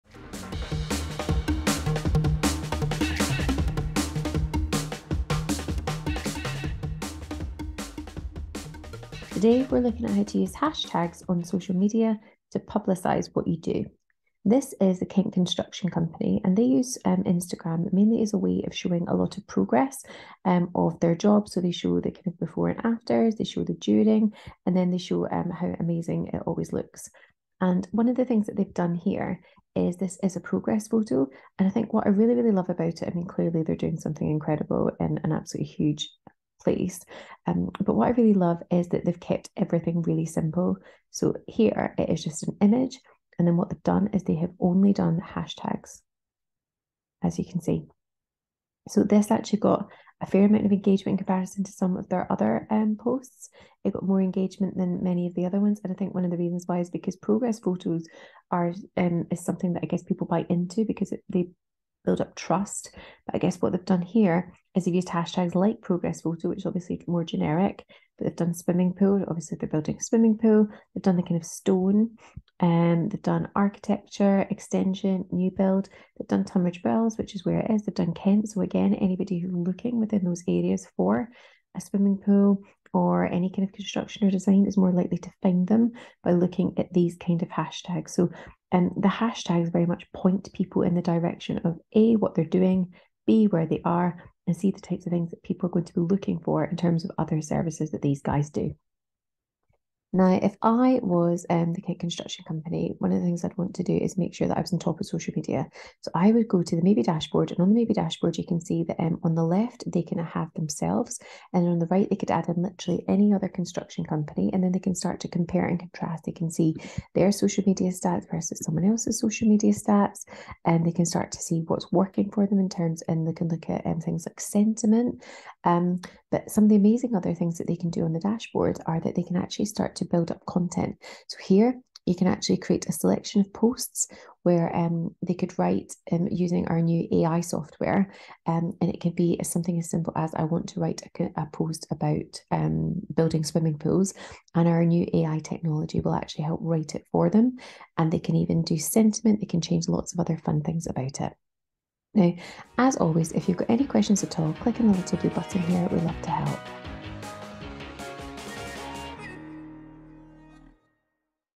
How to use progress pics on social media
Want to showcase your work via social media? Posting progress images, and before and after content, is a great way to do this, and it's a super engaging type of content too. Adding the right hashtags can boost your reach and engagement on these posts. Here we look at how a Kent construction company do this well.